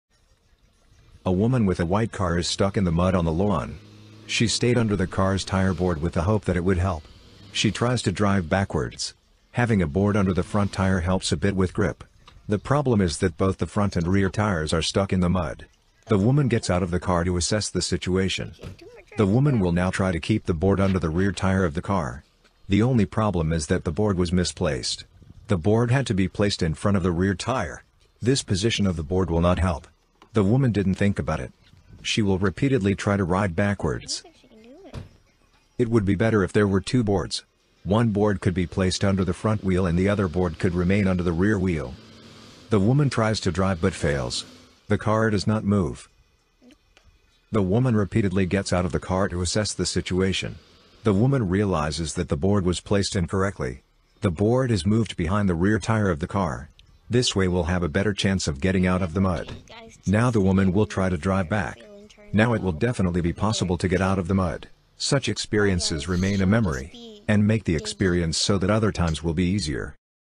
Women with white car stuck in mud - driving fails
Women driving fails. Women with white car stuck in mud and grass. Driving adventures. Pedal pumping in car. Funny mud stuck.